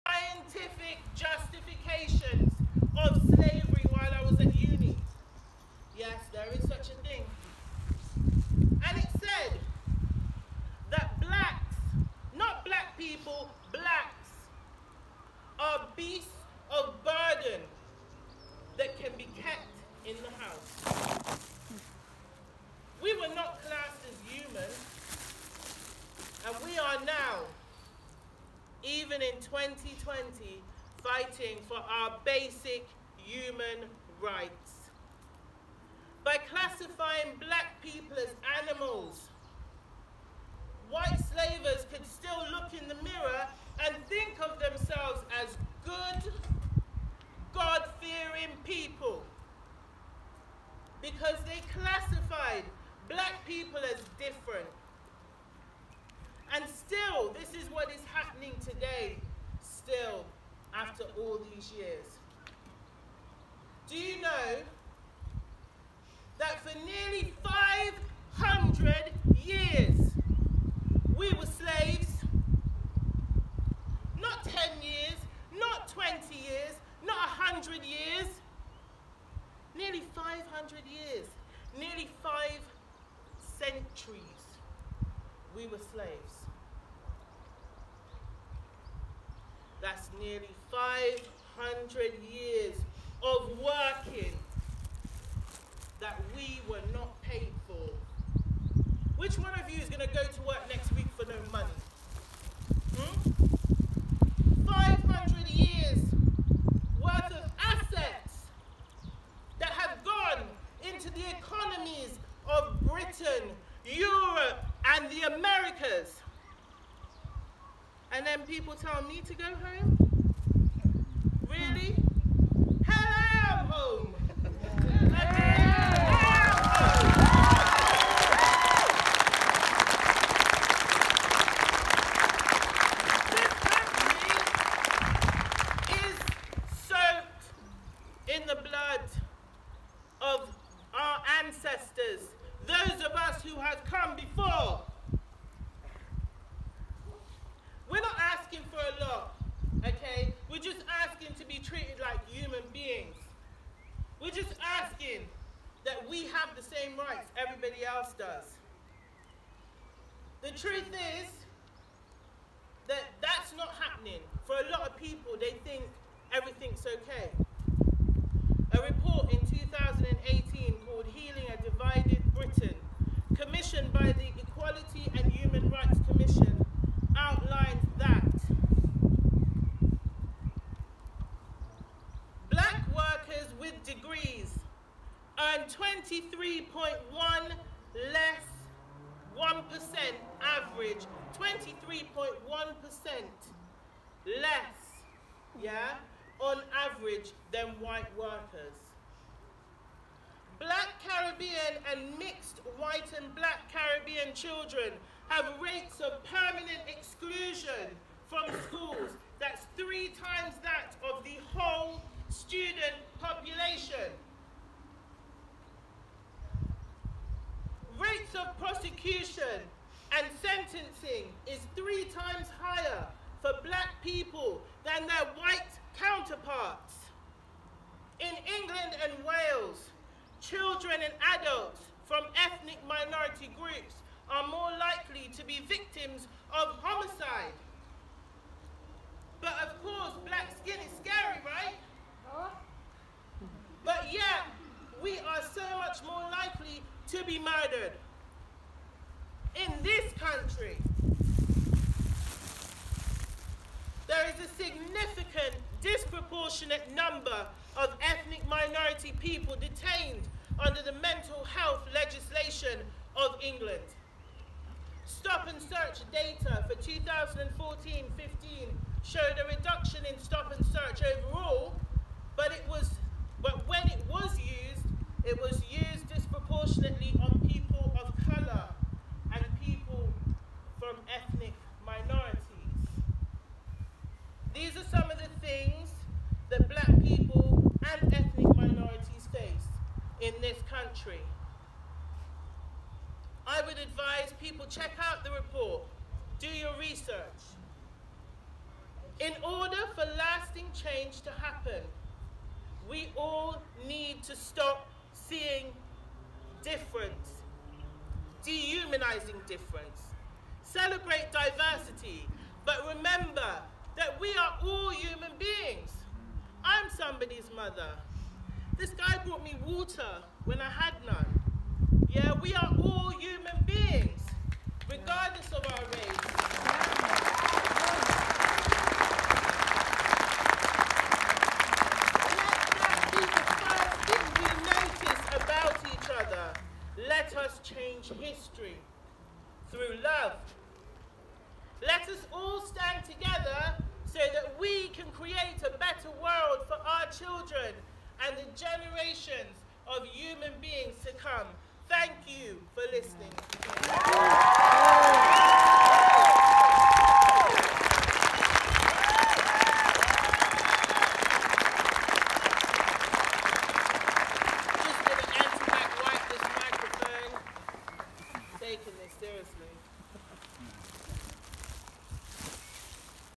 Video Cachelle Campbell at the Black Lives Matter demo in Trowbridge Park
Cachelle Campbell addresses the Black Lives Matters demo at Trowbridge Town Park on Thursday June 4.